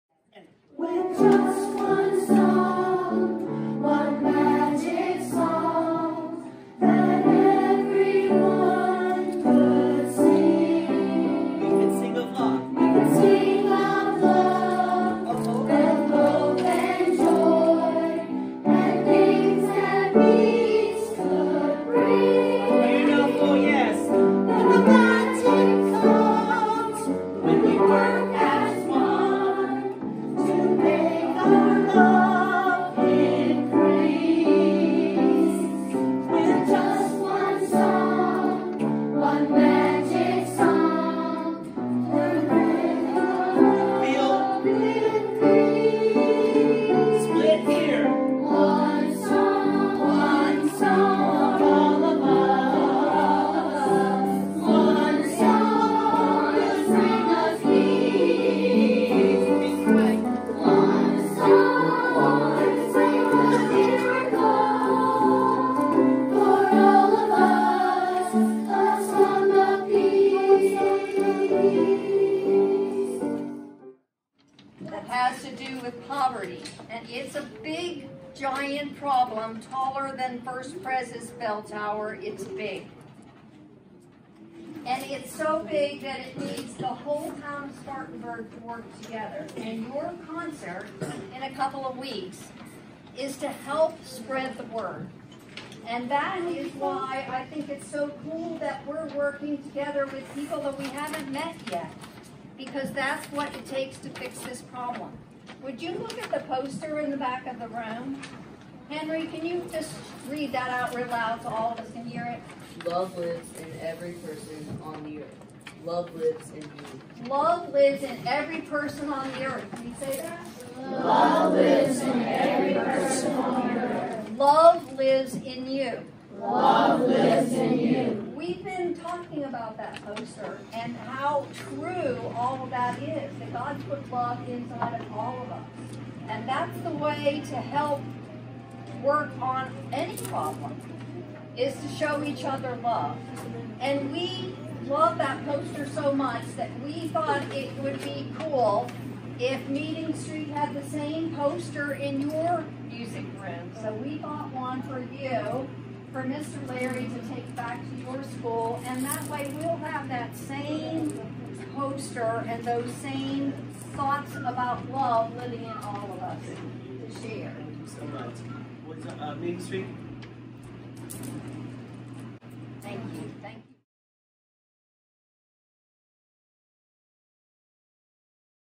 Concert of Hope Preview
Meeting Street Academy and First Presbyterian Church children's choirs rehearse together for the upcoming Concert of Hope to raise awareness about and funds to help those in poverty in Spartanburg. The concert will be held October 27, 2019 with Dr. Russell Booker, Superintendent of School District 7, as emcee.